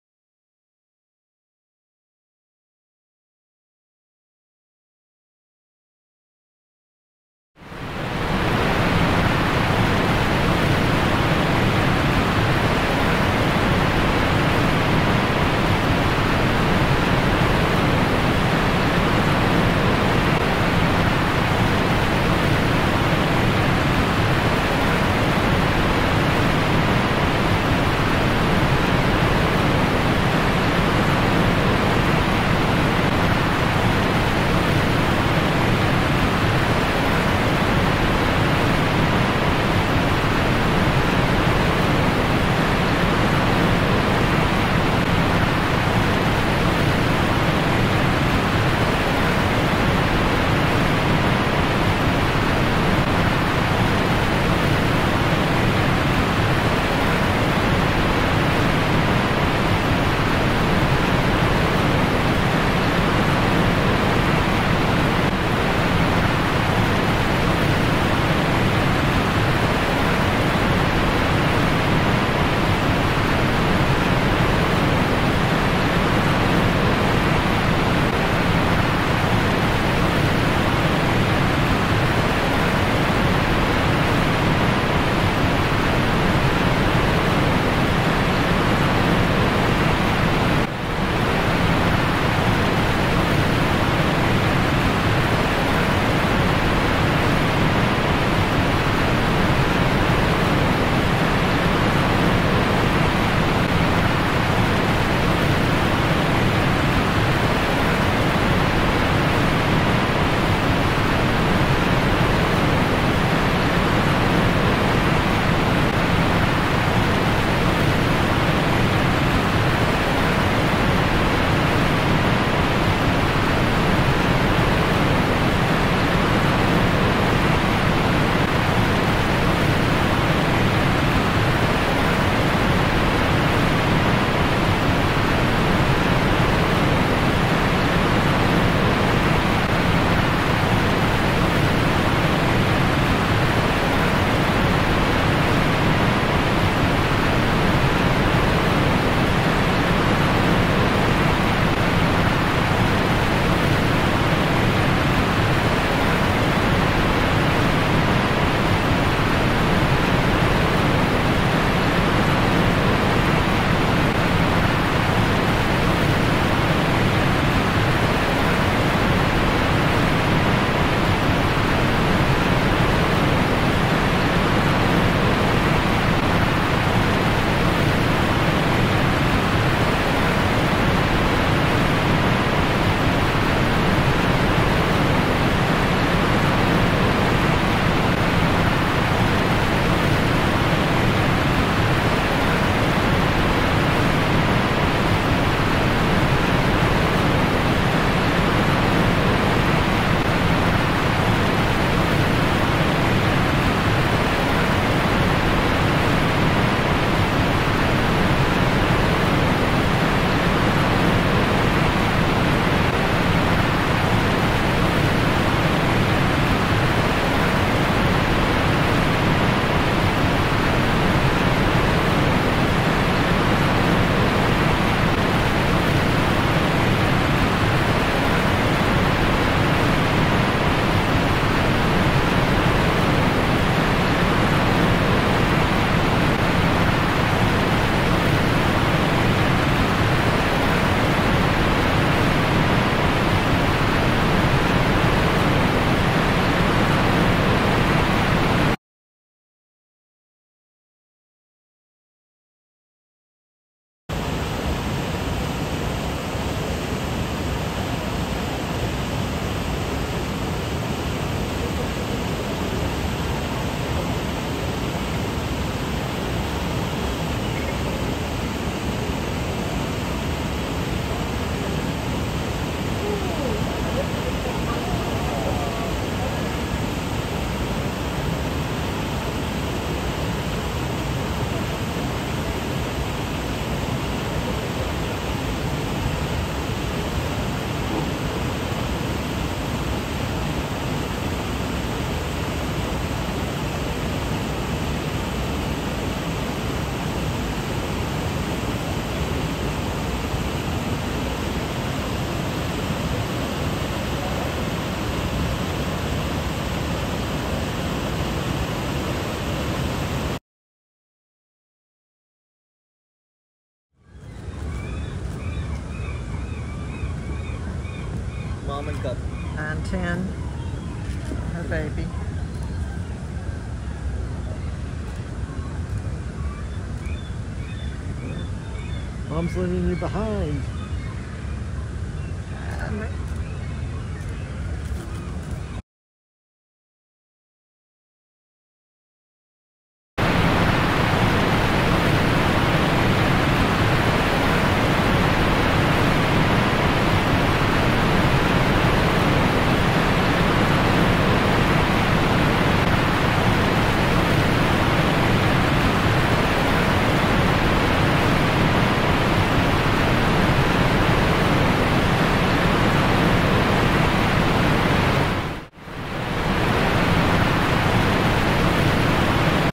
The Beady Families 909s 910s Eat Together 090522 HD
On September 5, 2022, 909 and and 910 were fishing on the lip.  909 made a catch and brought it down in front of the platform, where 909jr ultimately got the largest part of the fish, and the 910 and 910jr joined to share.  There are two videos in this reel of the same incident, just different viewpoints. In this video near the end at 5:40, there is a bear on the opposite bank that was not IDed when I created the video.  Chatters have now IDed this bear as most likely 284 Electra.